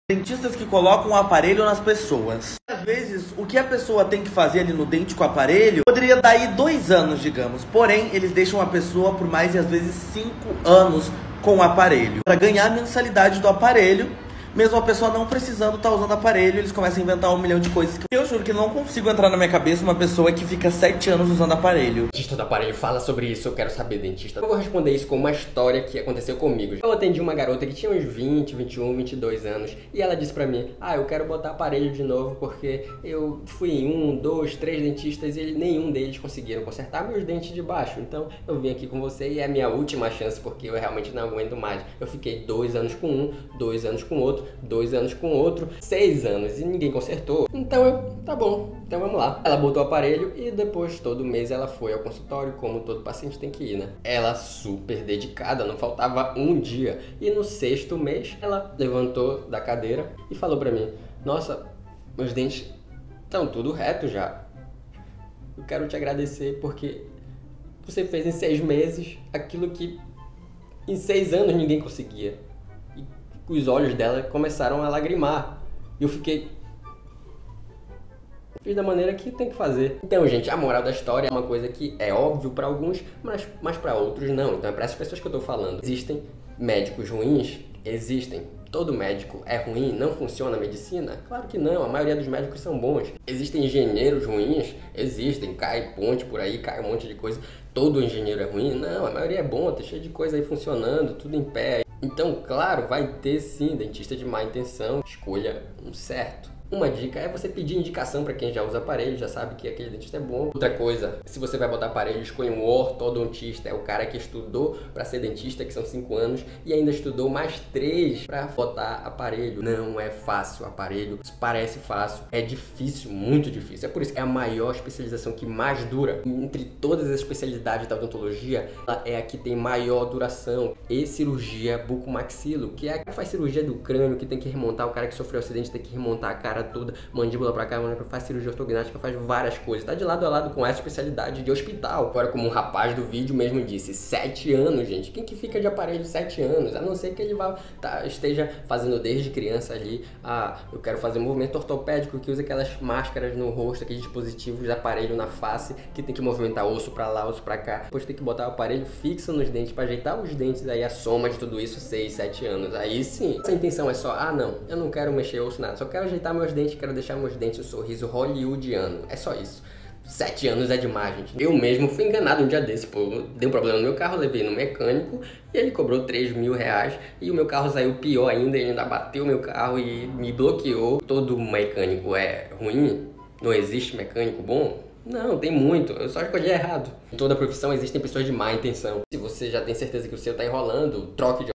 Ortodontista fala a Verdade sobre usar Aparelho por tanto tempo. Não devia falar isso mas…
Ortodontista fala a Verdade sobre quem fica muito Tempo de Aparelho. Dentista tá Enrolando?!